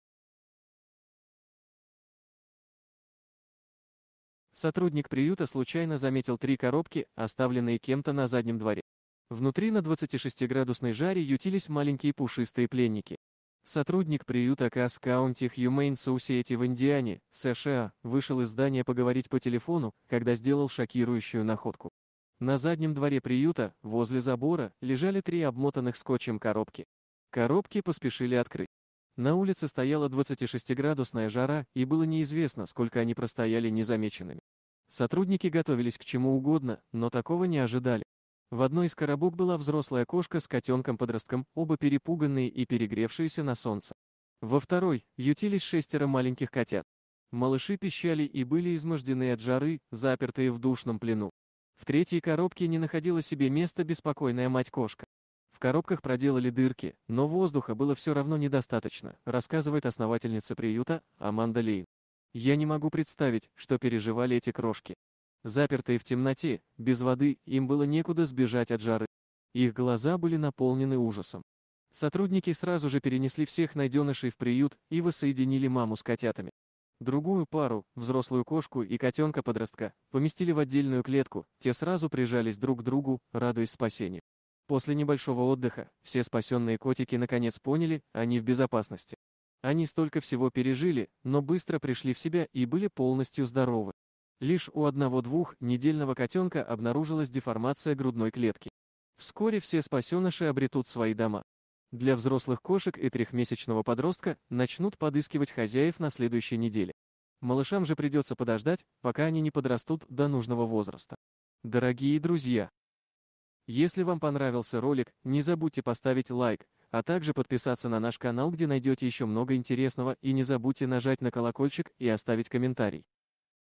Возле приюта бросили три обмотанных скотчем коробки, содержимое ужасает!
Сотрудник приюта случайно заметил три коробки, оставленные кем-то на заднем дворе. Внутри на 26-градусной жаре ютились маленькие пушистые пленники.